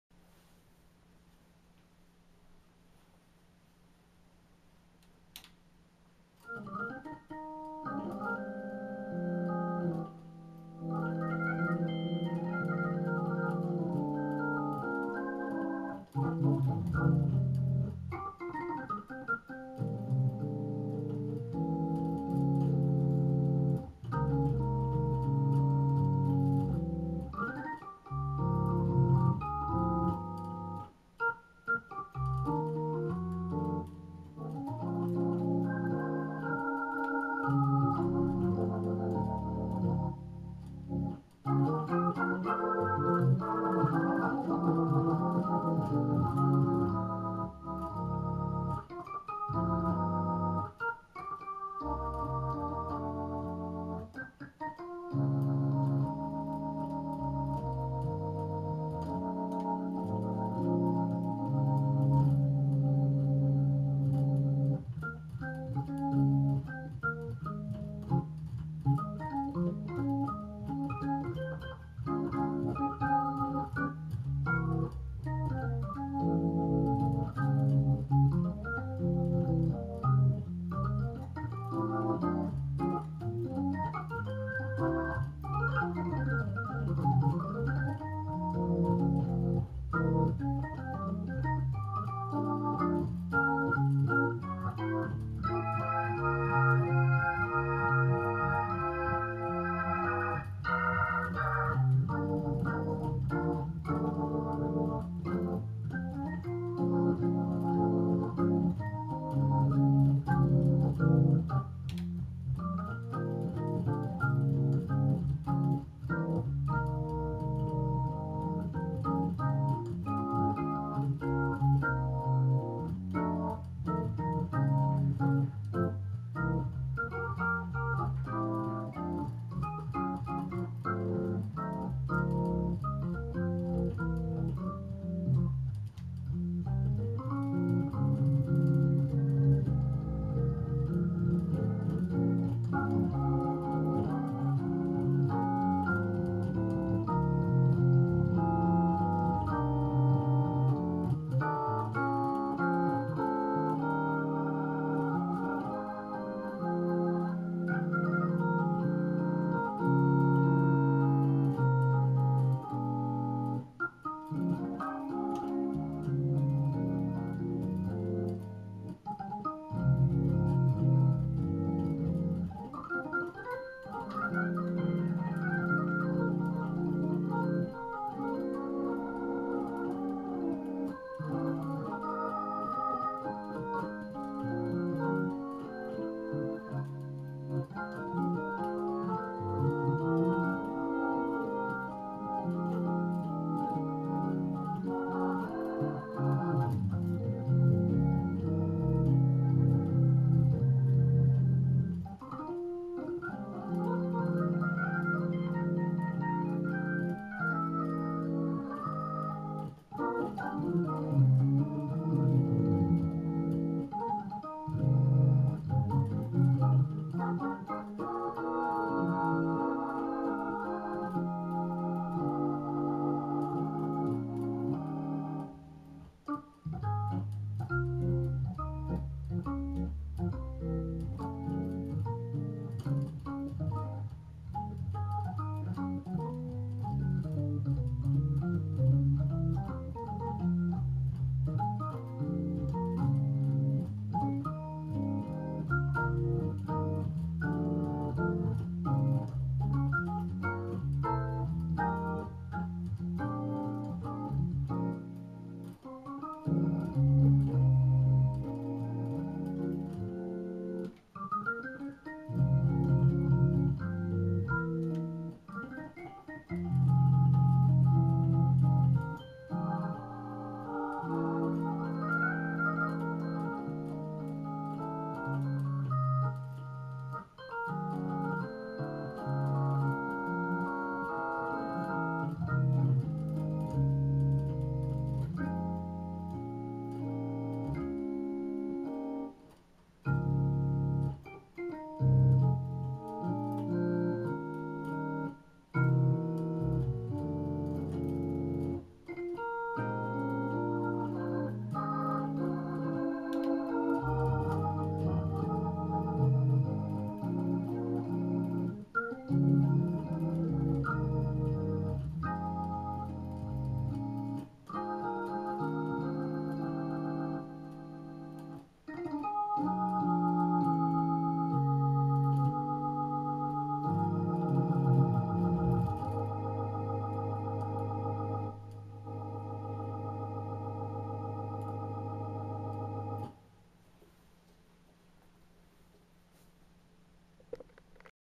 NORD Electro 3 - gospel / jazz #9
The 3rd in a series of 3 videos (#7, #8, #9) with a re-positioned keyboard amp for enhanced sound quality.  Again, there are no other 14# keyboards available on the market today, with both the excellent organ sounds as well as superb acoustic and electric piano sounds!  Also horns, strings, and percussive sounds are also available for download from the Nord site.